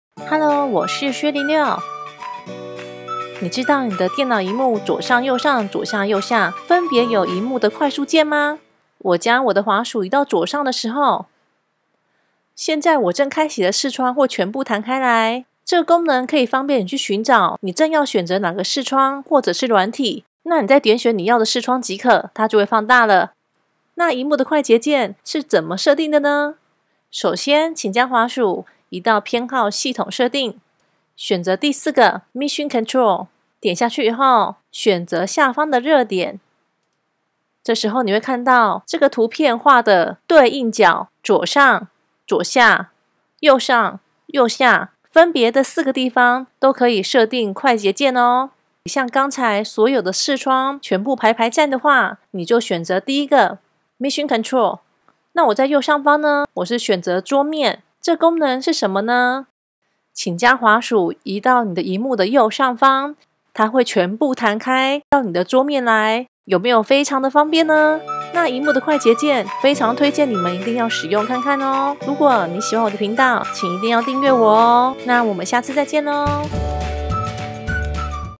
MAC課程🌟原來螢幕也有快速鍵？一個動作，視窗全部彈開！設定方式立刻學#macOS Sierra｜薛零六Bess Shiue
系統：macOS Sierra
關於mac新手必須知道的好用功能
螢幕的快捷鍵！
只要滑鼠一個“滑過去”的指令
就可以立刻彈開所有視窗囉！

(可開啟CC字幕：中文)

嗨，我是薛零六（X06）
如果你喜歡我的頻道、並給我支持鼓勵的話
goo.gl/Dkg1ew